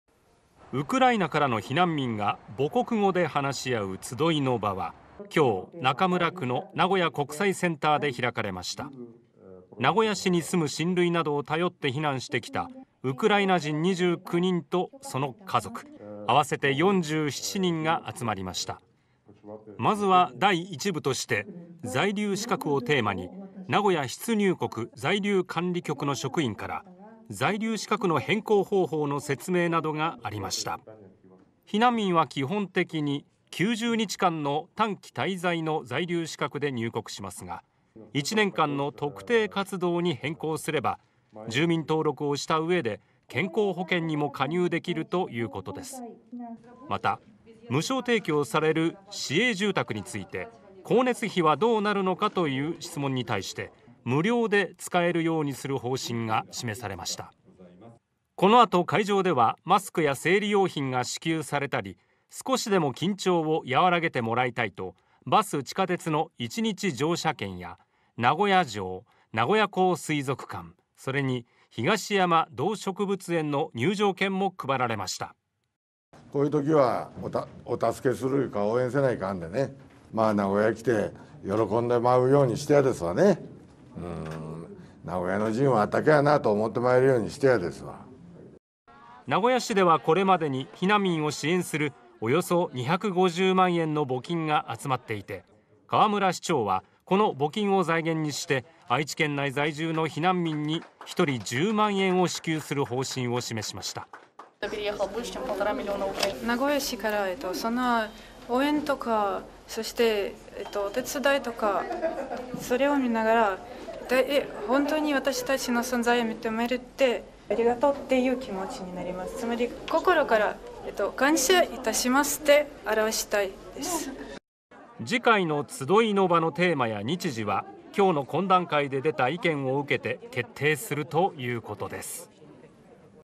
ウクライナ避難民や家族が集まる 在留資格の変更方法など説明 愛知県内１人1０万円支給も 名古屋市 (22/04/08 19:02)
ウクライナからの避難民が、母国語で話し合う「つどいの場」が、名古屋市で開催されました。どんな支援がいま必要だと考えられているのでしょうか？

　ウクライナからの避難民が母国語で話し合う「つどいの場」は、８日中村区の名古屋国際センターで開かれました。

　名古屋市に住む親類などを頼って、避難してきたウクライナ人２９人とその家族、あわせて４７人が集まりました。

　まずは第１部として「在留資格」をテーマに、名古屋出入国在留管理局の職員から在留資格の変更方法の説明などがありました。

　避難民は基本的に９０日間の「短期滞在」の在留資格で入国しますが、１年間の「特定活動」に変更すれば、住民登録をした上で健康保険にも加入できるということです。

　また、無償提供される名古屋の市営住宅について、「光熱費はどうなるのか？」という質問に対して、無料で使えるようにする方針が示されました。

　このあと会場ではマスクや生理用品が支給されたり、少しでも緊張を和らげてもらいたいと、バス・地下鉄の１日乗車券や、名古屋城・名古屋港水族館、それに東山動植物園の入場券も配られました。

　（名古屋・河村たかし市長）
　「こういうときは応援しないと。名古屋に来て喜んでもらえるように、名古屋人は温かいと思ってもらえるようにしたい」。

　（ウクライナからの避難民　エリザベータさん・２２歳　通訳音声）
　「名古屋市から支援をしてもらい、私たち（避難民）の存在を認めてもらい、ありがとうという気持ちになりました。心から感謝いたします」

　これまでに避難民を支援する、およそ２５０万円の募金が集まっていて、河村市長はこの募金を財源にして、愛知県内在住の避難民に１人１０万円を支給する方針を示しました。

　次回の「つどいの場」のテーマや日時は、８日の懇談会で出た意見を受けて決定するということです。